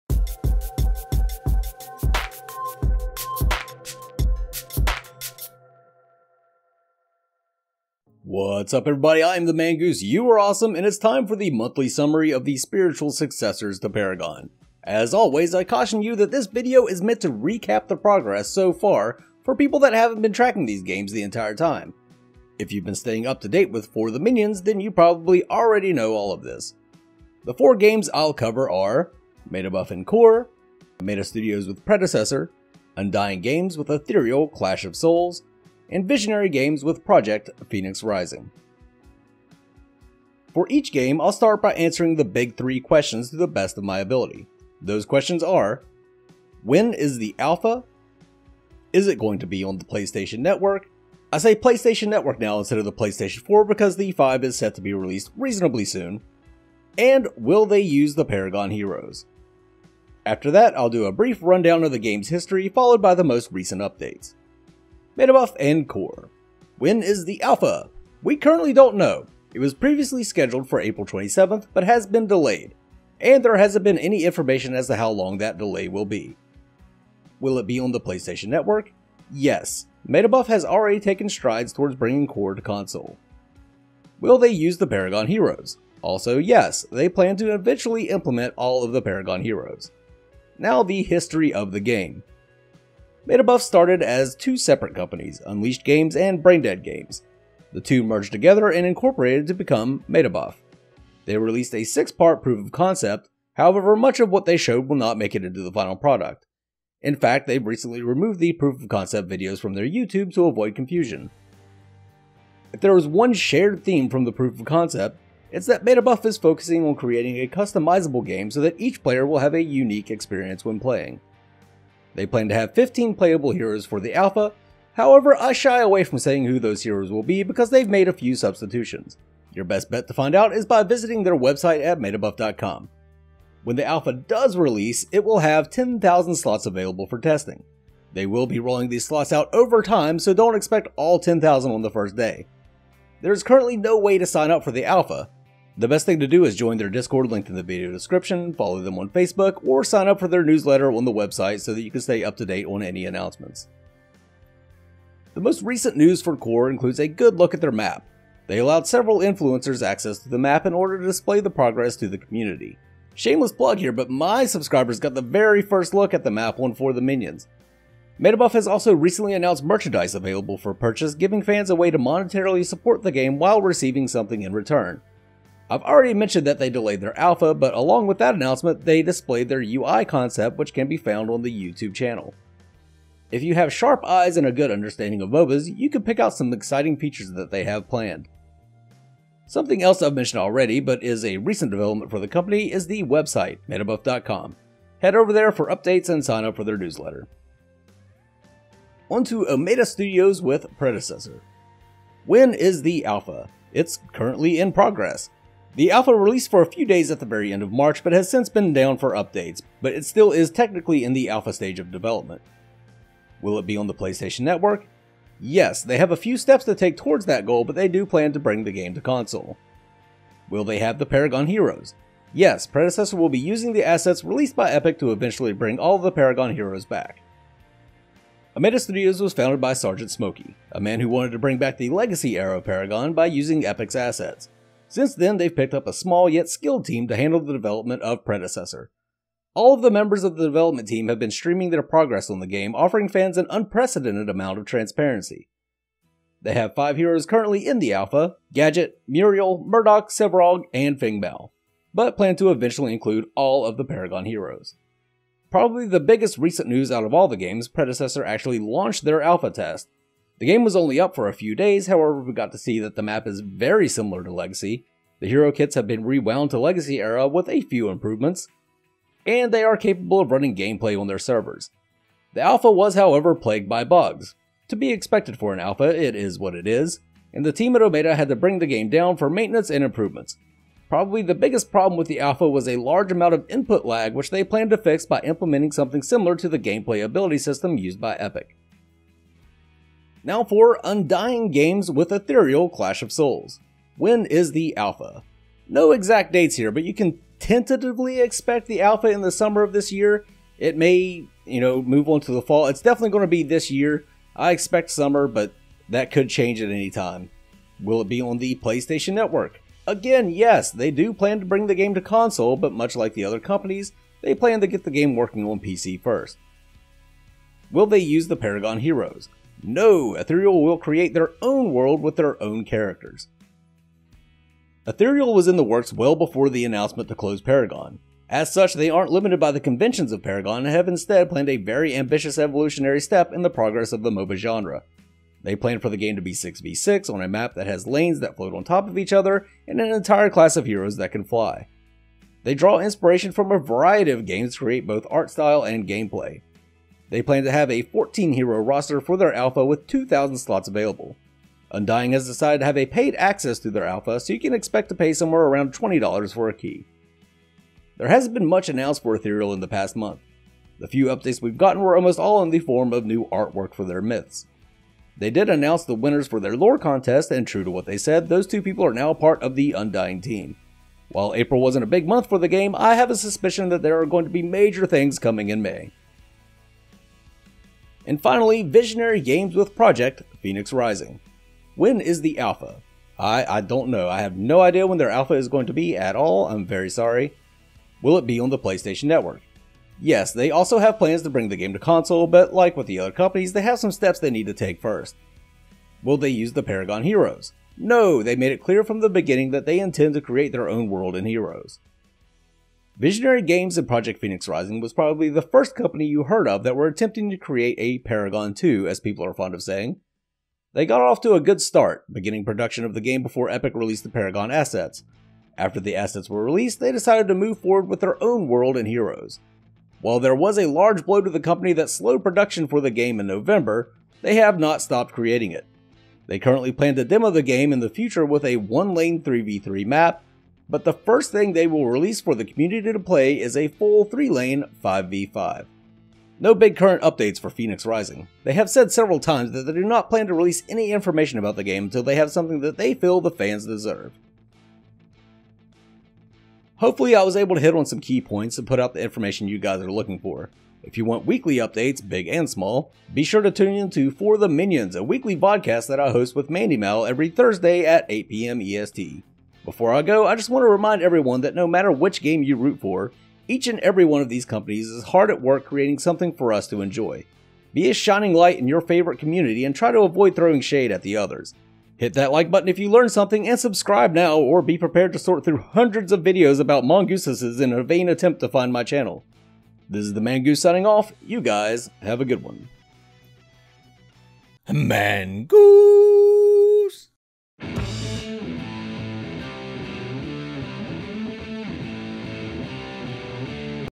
The Road Back to Paragon: April summary of the games
Continuing my monthly roll up of what's been going on so far with the spiritual successors to Paragon.

Metabuff Website - Metabuff.com